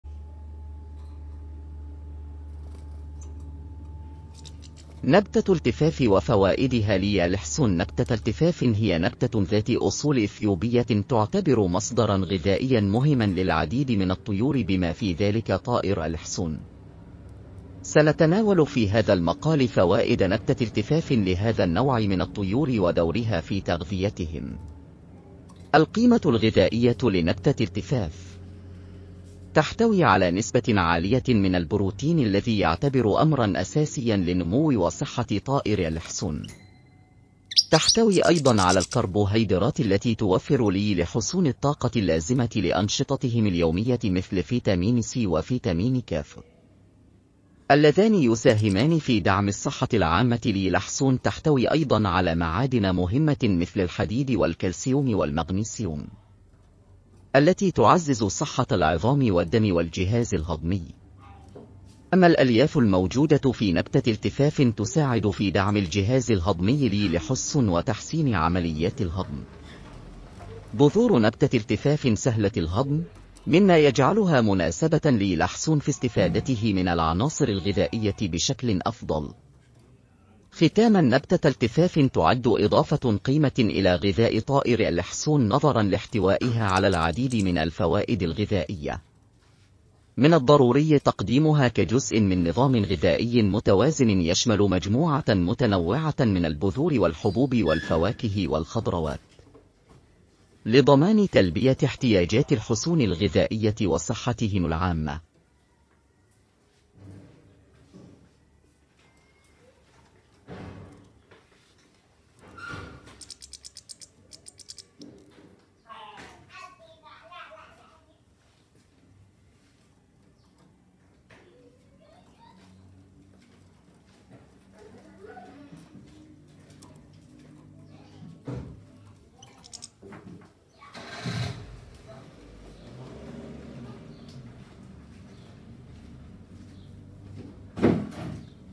فوائد التيفاف
‎هل ترغب في معرفة المزيد عن نبتة التيفاف وكيف يمكن أن تحدث فرقًا في صحة طيور الحسون؟ في هذا الفيديو، سنستكشف فوائد هذه النبتة الغذائية الرائعة، وكيف يمكن لبذور التيفاف أن تلعب دورًا مهمًا في تحسين تغذية طيور الحسون. سنتناول أيضًا كيفية تضمينها في نظام غذائي متوازن لتعزيز صحة هذه الطيور الرائعة.